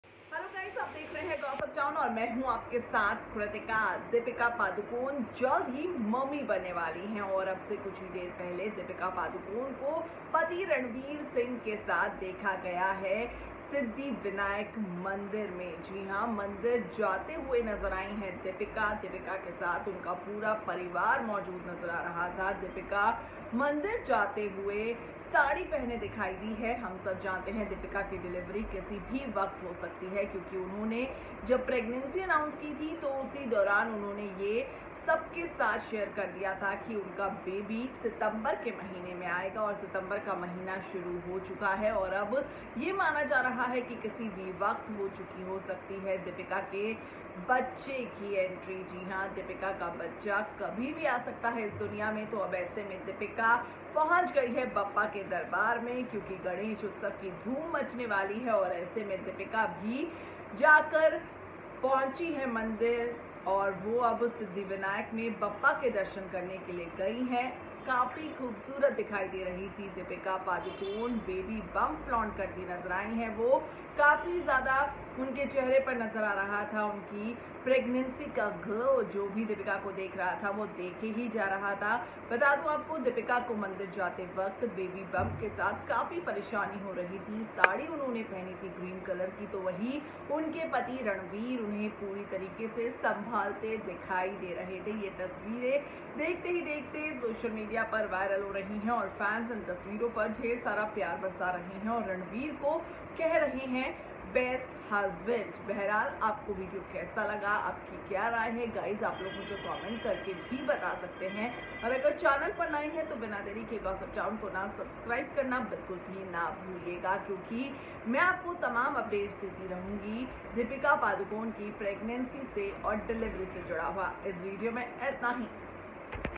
Pregnant Deepika Padukone seeks blessings from Siddhivinayak temple with husband Ranveer Singh
Deepika Padukone flaunts baby bump at siddhivinayak temple. Watch the video to know more about this.